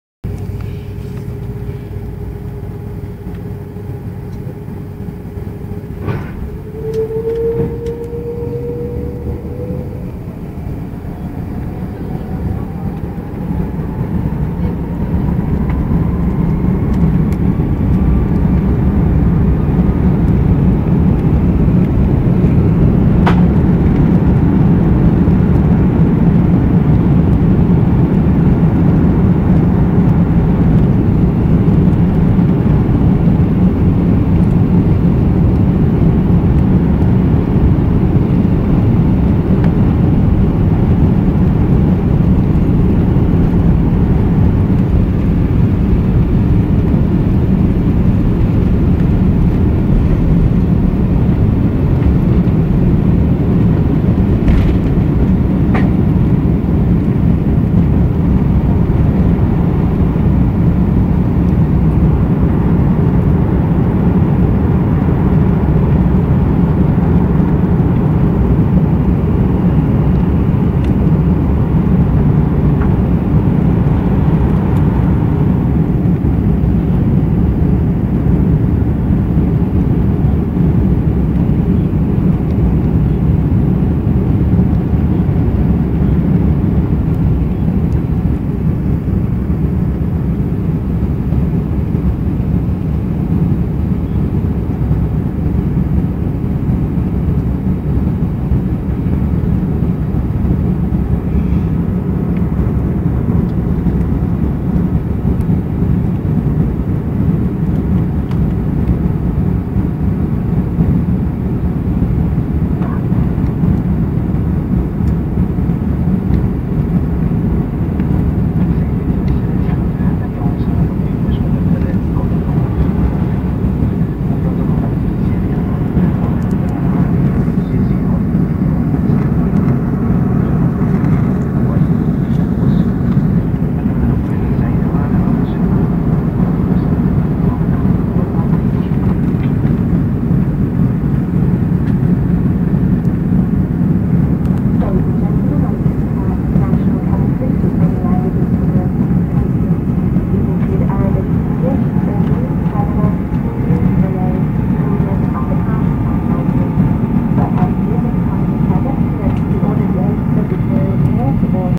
A330 Takeoff
emirates takes-off from Dubai in the morning heading for Delhi